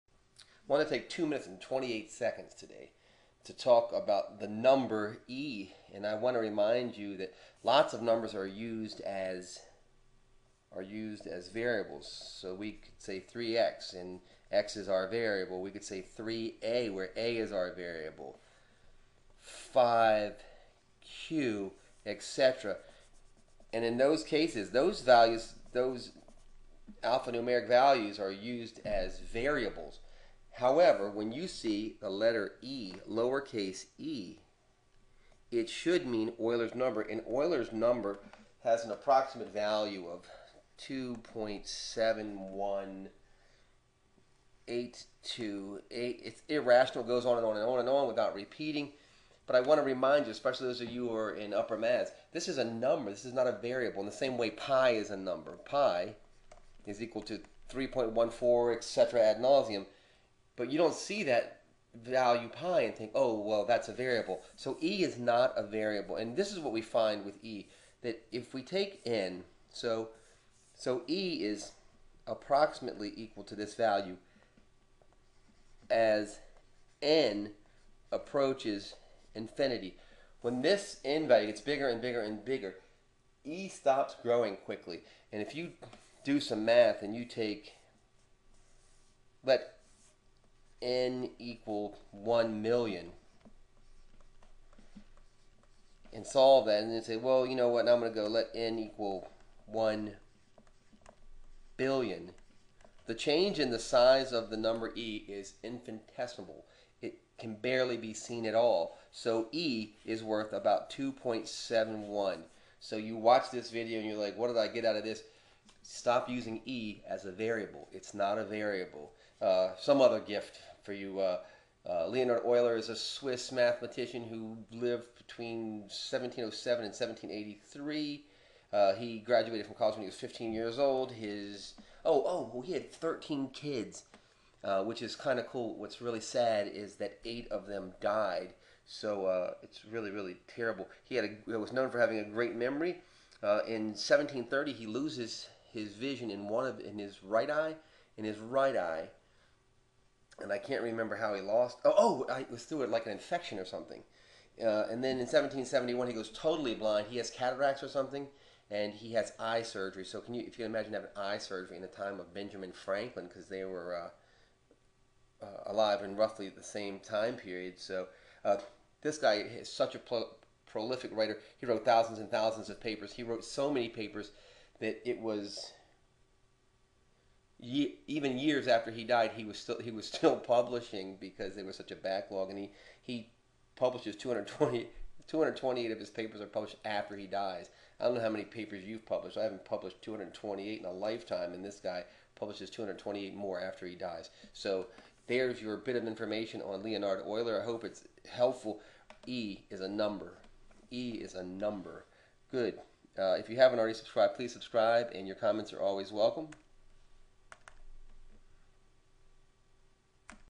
The Number e
A QUICK EXPLANATION OF THE NUMBER E.  This video will really help you understand